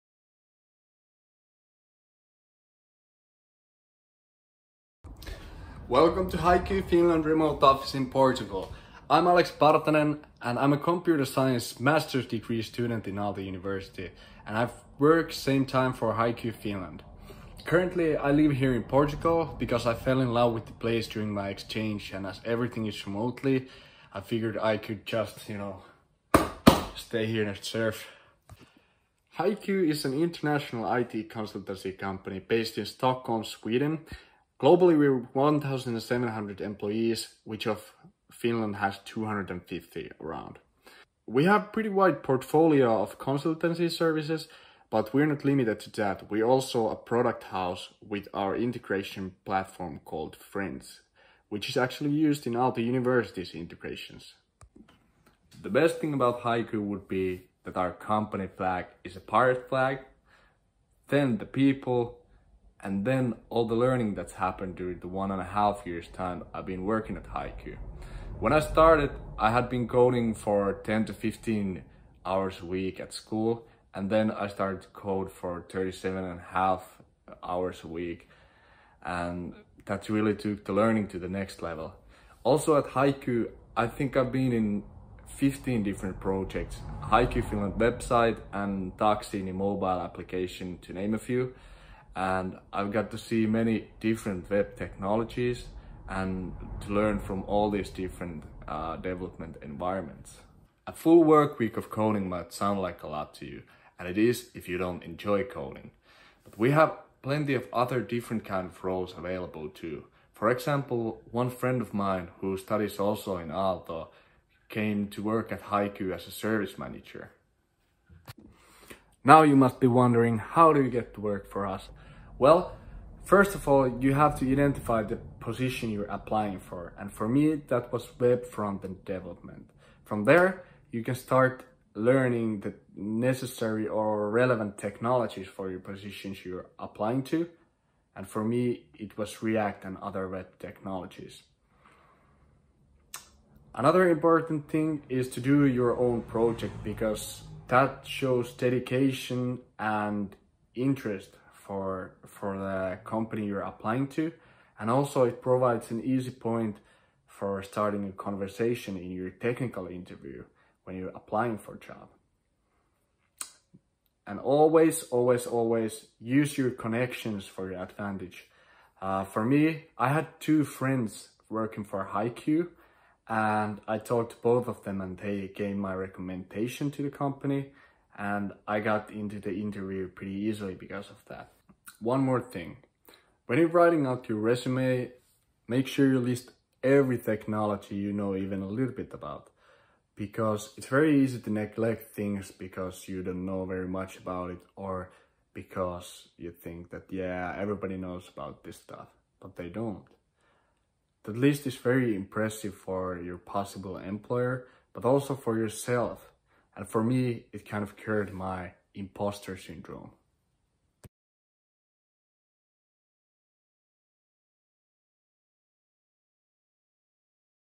Working at HiQ & Frends
Working in HiQ as a junior developer and how to score your first developer job.